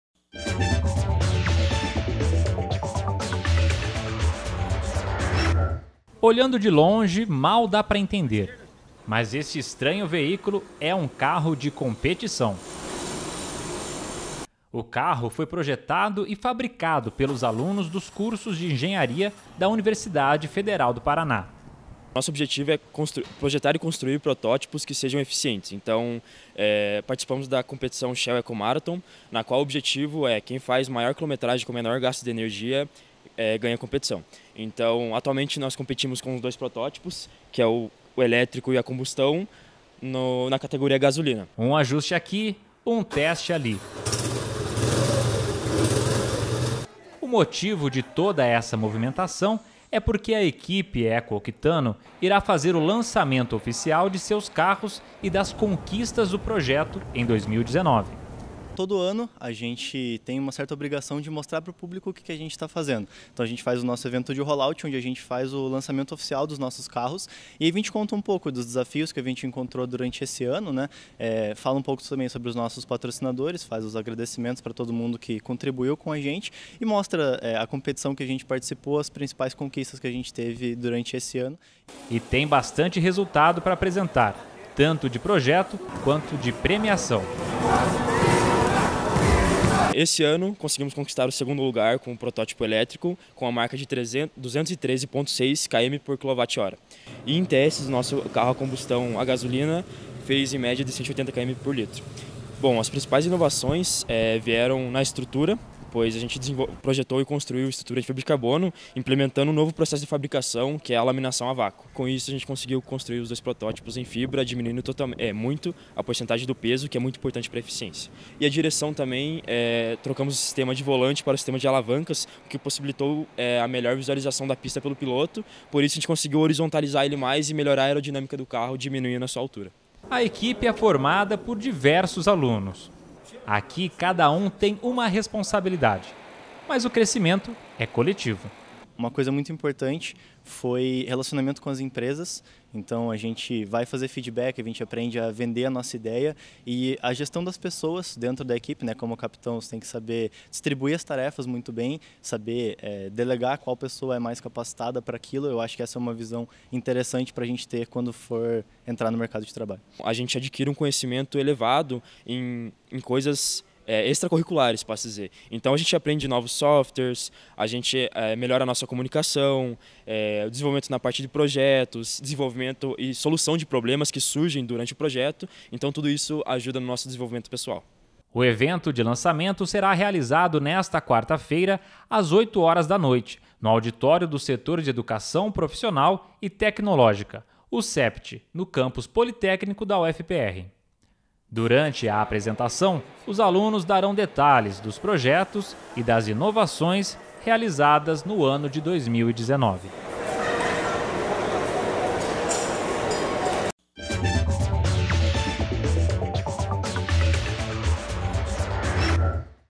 Informa | ECO Octano
Na UFPR, alunos de engenharia encararam o desafio de projetar e construir um veículo com maior eficiência energética. O grupo Eco Octano fez o dever de casa e apresenta oficialmente seu vitorioso carro nesta quarta, 06/11, às 20h, no Auditório do Sept - Setor de Educação Profissional e Tecnológica. O evento é aberto a toda comunidade.